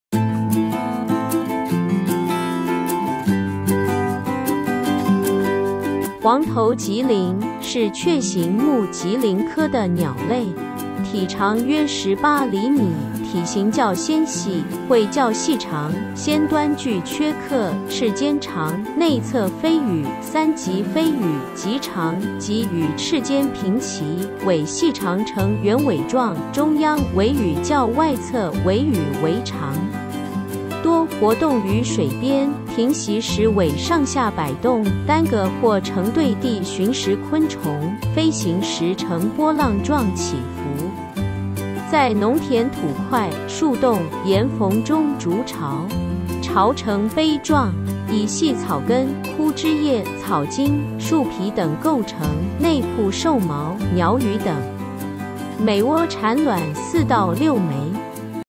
早安 黃頭鶺鴒
黃頭鶺鴒，是雀形目鶺鴒科的鳥類。體長約18釐米，體型較纖細，喙較細長，先端具缺刻;翅尖長，內側飛羽(三級飛羽)極長，幾與翅尖平齊;尾細長呈圓尾狀，中央尾羽較外側尾羽為長。多活動于水邊，停息時尾上下擺動，單個或成對地尋食昆蟲，飛行時呈波浪狀起伏。在農田土塊、樹洞、岩縫中築巢。巢呈杯狀。以細草根、枯枝葉、草莖、樹皮等構成，內鋪獸毛，鳥羽等。每窩產卵4~6枚。夏季食物主要是昆蟲，秋季兼食些草籽;為地棲鳥類，喜沼澤草甸、苔原帶及柳樹叢。